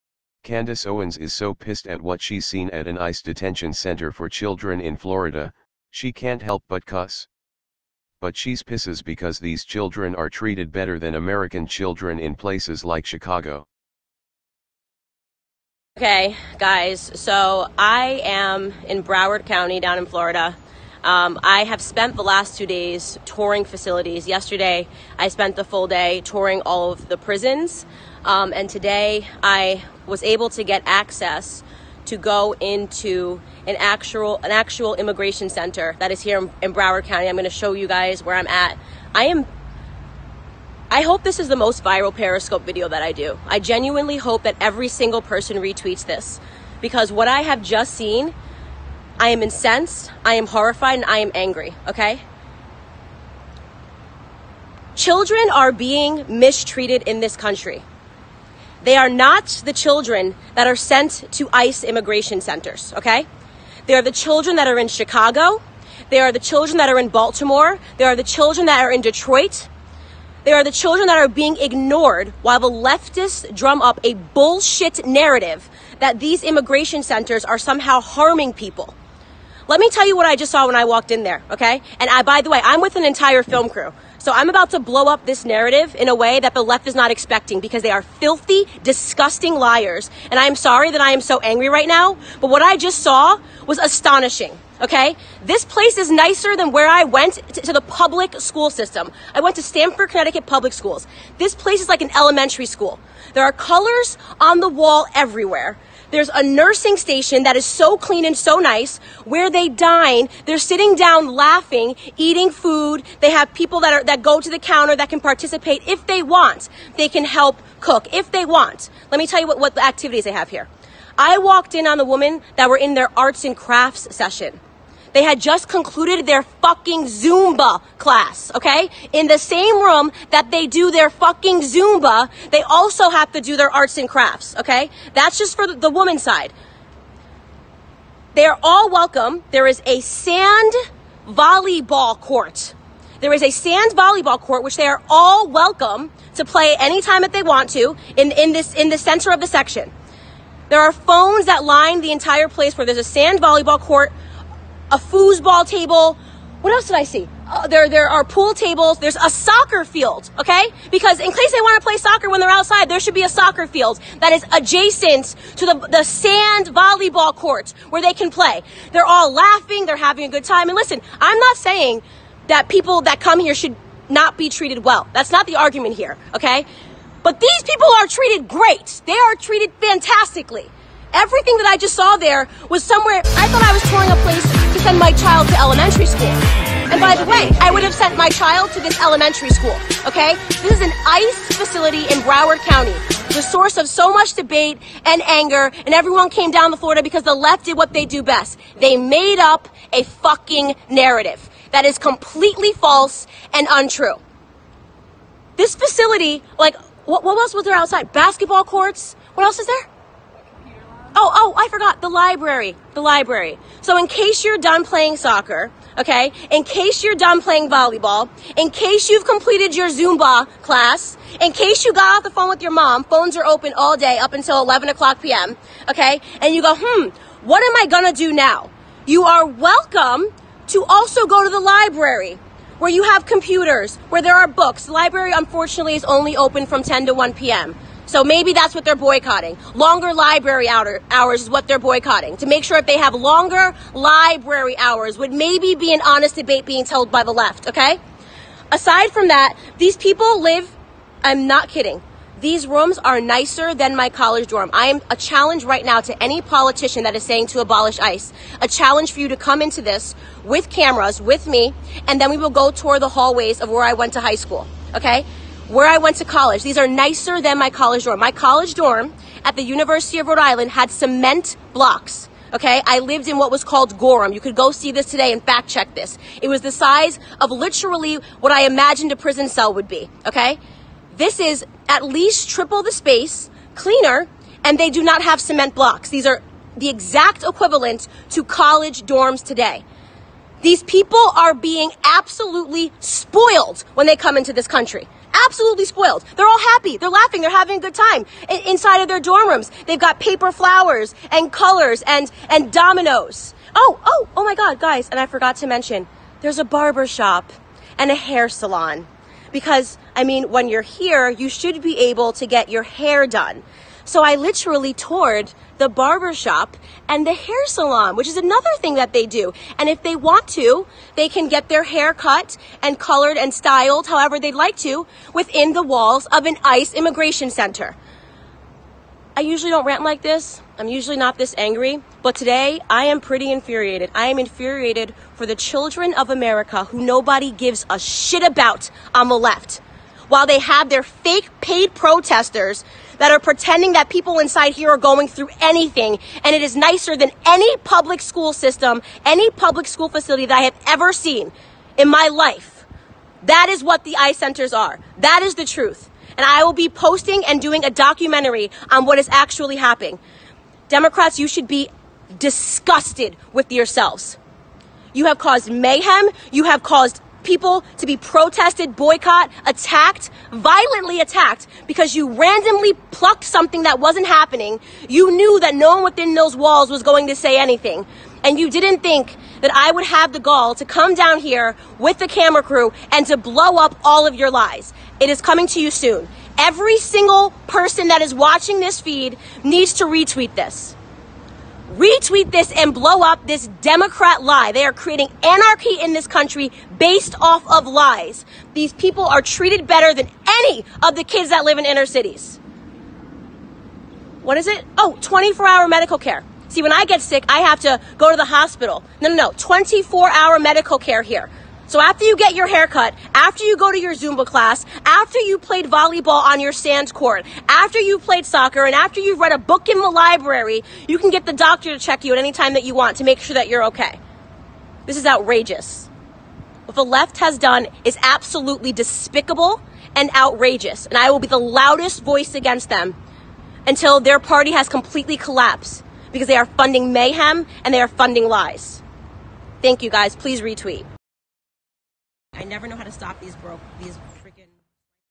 Candace Owens Visits ICE.
Candace Owens seen at an ICE detention center for children in Florida, she can’t help but cuss! But she’s ticked off because these children are treated better than American children in places like Chicago!